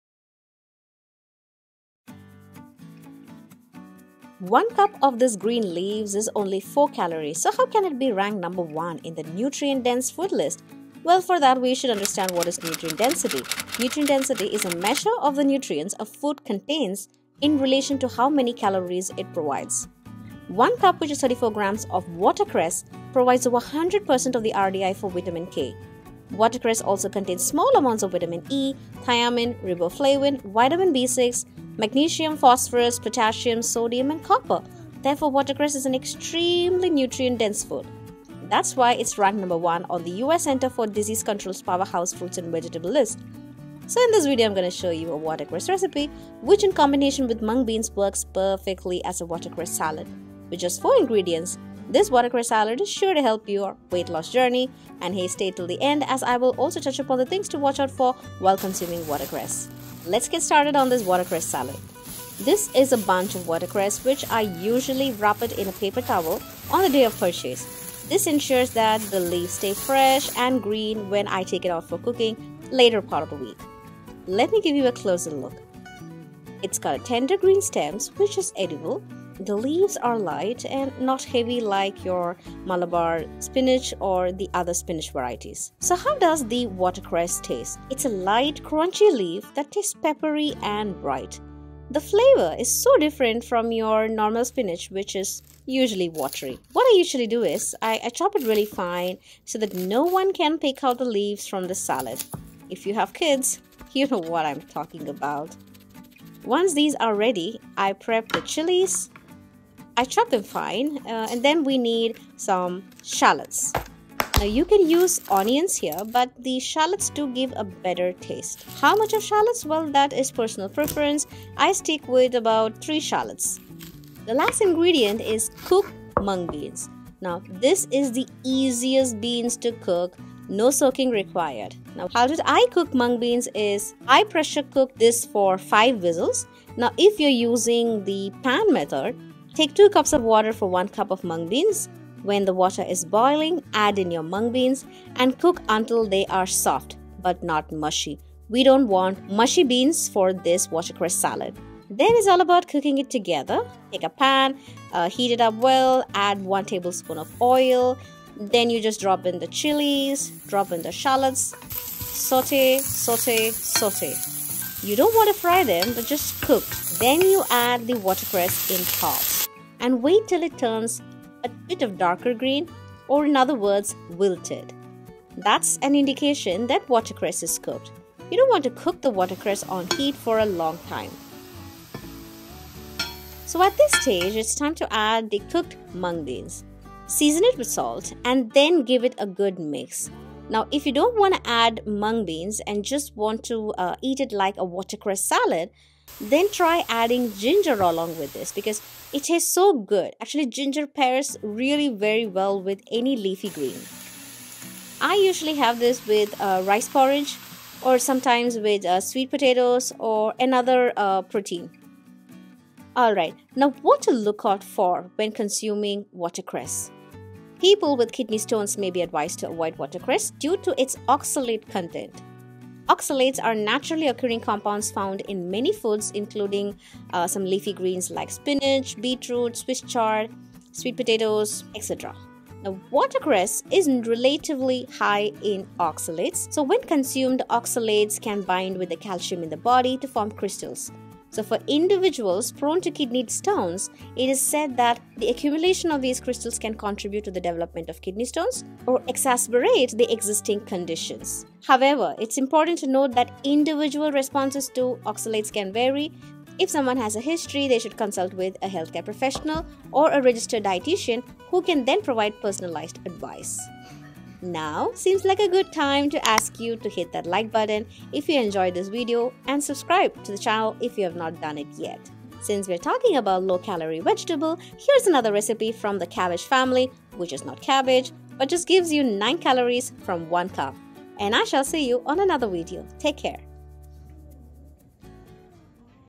This Food Is Ranked No 1 In Nutrient Dense Foods
Discover the #1 ranked nutrient dense food .With 14 gm of protein in this wholesome family meal idea ,this weight loss salad will help you lose that weight.

Rate the video! 👍 - I will be very happy and it will help to grow the channel.

🔵TIMESTAMPS:
0:00 Calories Information
0:21 which food is ranked 1 in nutrient dense food list
1:00 how to make this weight loss salad
1:25 How does it taste
1:47 Ingredients
2:13 how to cook mung beans
2:33 cooking the weight loss salad
3:33 who should not eat this food
3:53 and why ?

Looking for a healthy and delicious meal that is ranked number 1 in nutrient-dense foods? Look no further than this watercress salad recipe! Not only is it packed with vitamins and minerals, but it's also a great option for those looking to lose weight. Try it out and see for yourself!
 So in this video I am going to show you A watercress recipe which in combination with Mung beans works perfectly as a watercress salad. With just 4 ingredients ,  this watercress salad is sure to help your weight loss journey. And hey stay till the end , as I will also touch upon the things to watch out for while consuming watercress.

Healthline website,